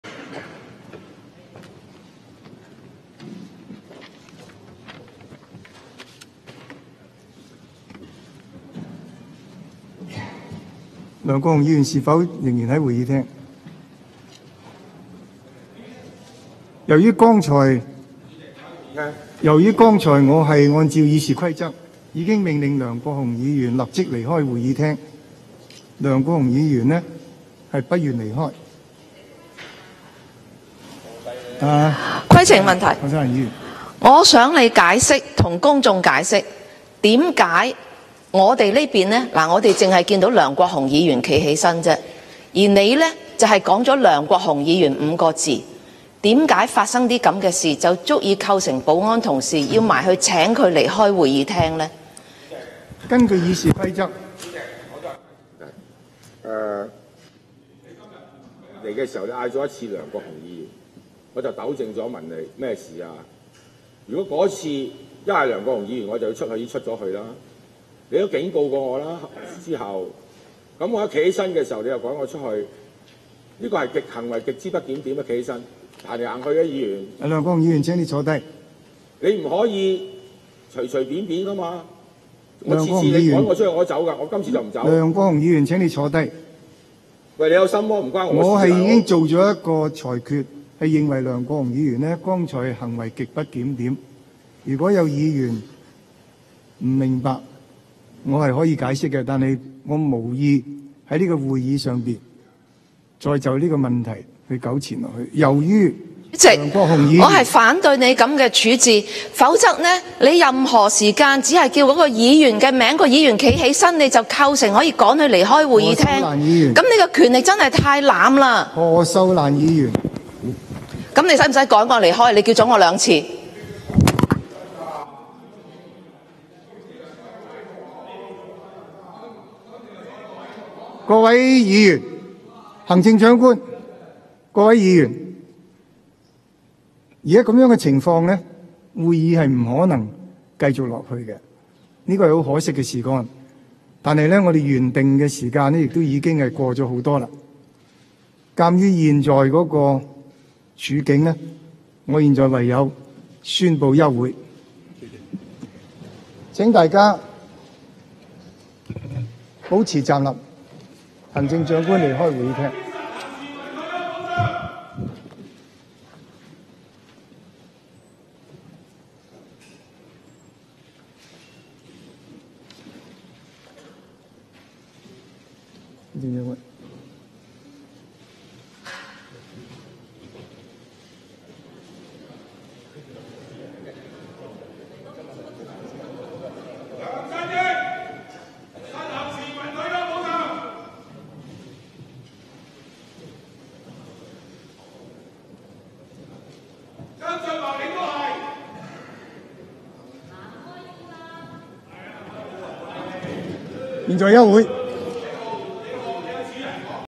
曾鈺成驅逐站立的長毛梁國雄出會議廳 長毛不滿主席隨便驅逐拒絕離開
約10:45am
會議重新開始後，長毛梁國雄拒絕離開會議廳，何秀蘭不滿主席曾鈺成叫長毛名字後長毛站立而驅逐他出會議廳； 長毛解釋其不滿，拒絕離開，最後主席被迫休會